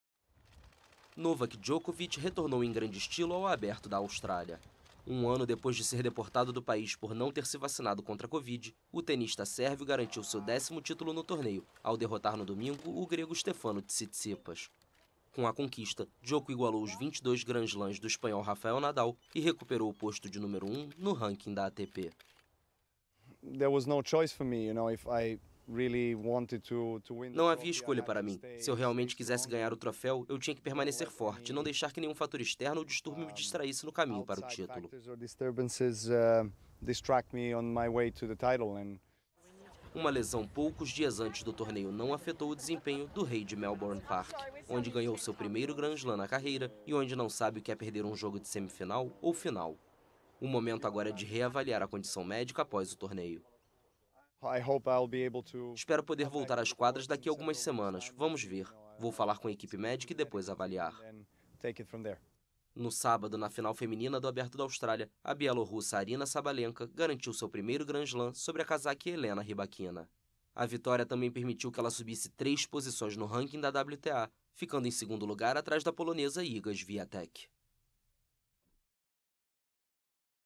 Após título na Austrália, Djokovic volta a ser número 1 do mundo | AFP
Campeão pela décima vez do Aberto da Austrália no domingo, o sérvio Novak Djokovic voltou a ser o tenista número 1 do mundo, de acordo com o ranking publicado pela ATP nesta segunda-feira.